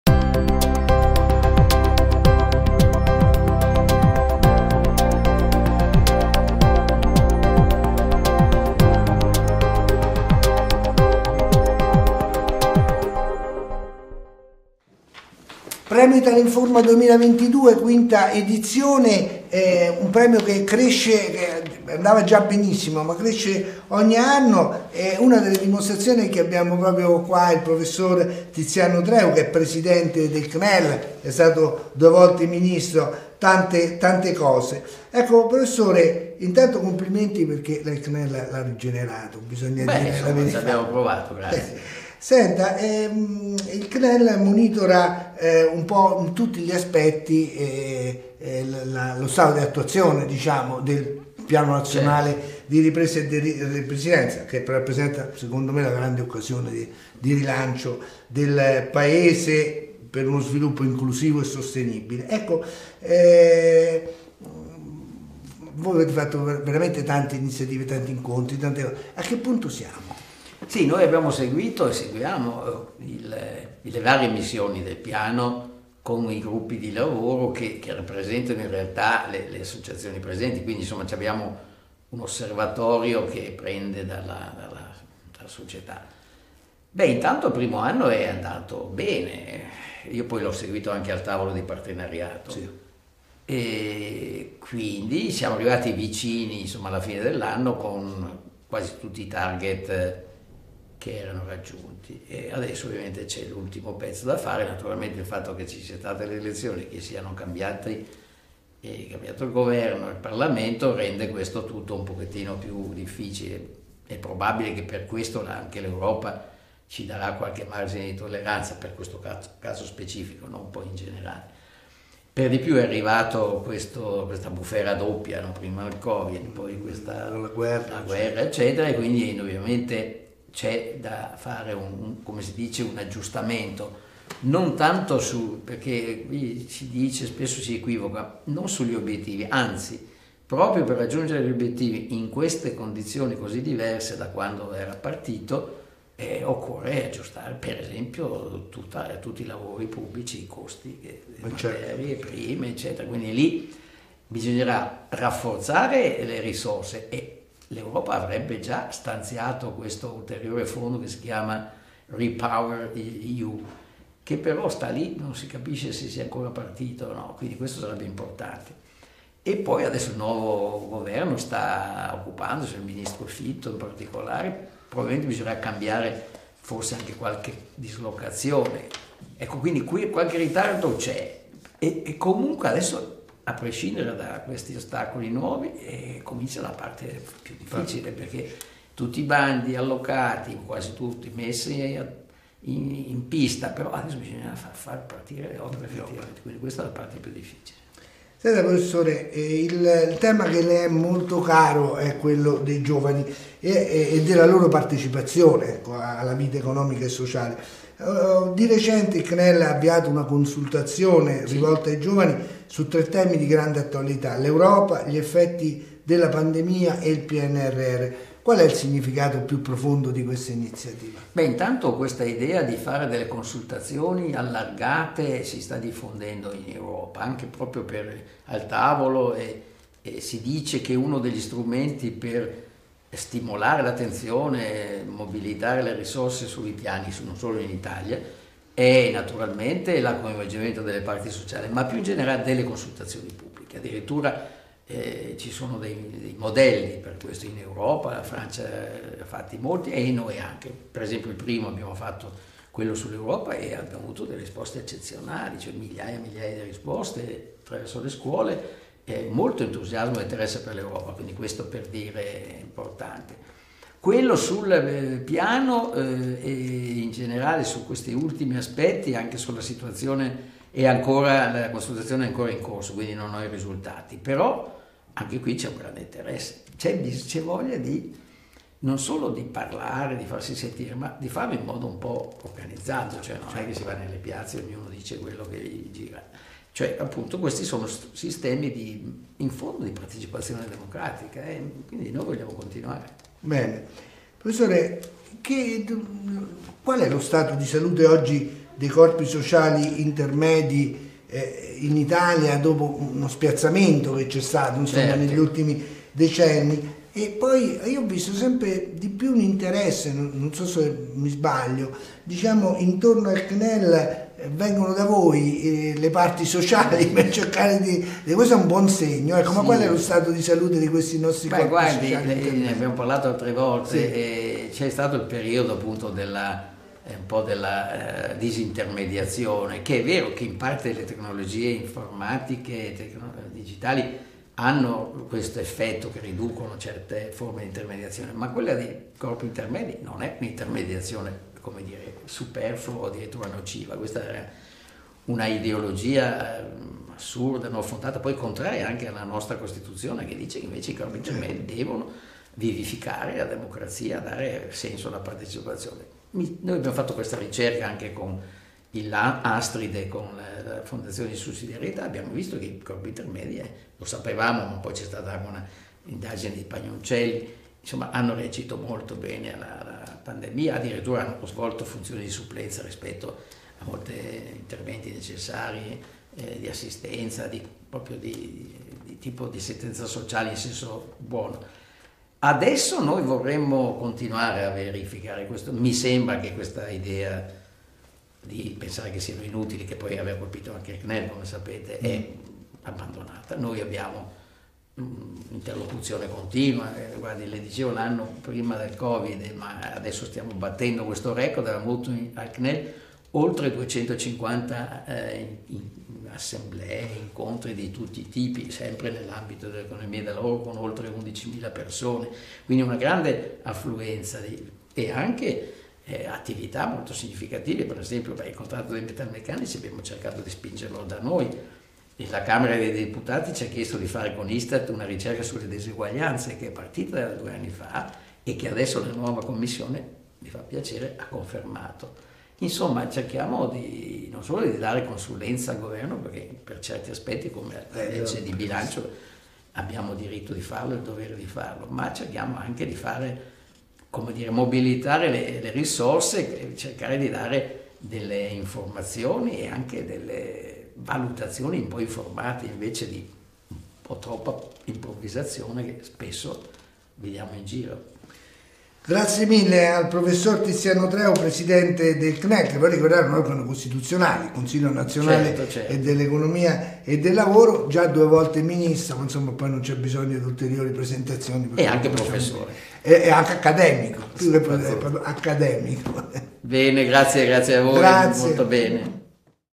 Premio Italia Informa 2022 - Intervista al Prof. Tiziano Treu, Presidente del Cnel
E’ giunto alla quinta edizione il "Premio Italia Informa" che celebra le eccellenze del Paese. Il riconoscimento è rivolto a personalità operanti in ambiti diversi, che si sono particolarmente distinte durante il proprio percorso professionale e di vita. Il 26 novembre 2022, presso il Rome Cavalieri Waldorf Astoria Hotel, sono stati premiati: Eugenio de Blasio, founder e Ceo di Green Arrow Capital; Alfredo Goldaniga, founder e Ceo di Pcmi spa; Gianmatteo Manghi, Ceo Cisco Italy; Flavia Mazzarella, Presidente Bper Banca; Michele Crisci, presidente e Amministratore delegato Volvo Car Italia e Presidente Unrae, l’associazione delle Case automobilistiche estere che operano in Italia; Leonardo Massa, Managing director MSC Crociere; Francesco Starace, Amministratore delegato Enel (che si è collegato via streaming da Londra). Per le categorie speciali, il premio per il Personaggio dell’anno è stato assegnato a Elisabetta Belloni, Direttore Generale Dis; quello alla carriera a Maria Bianca Farina, Presidente Ania; quello per l’economia a Tiziano Treu, Presidente del Cnel. Al senatore Adolfo Urso, ministro delle Imprese e del Made in Italy, è stato assegnato il premio “Rinascimento Italia”.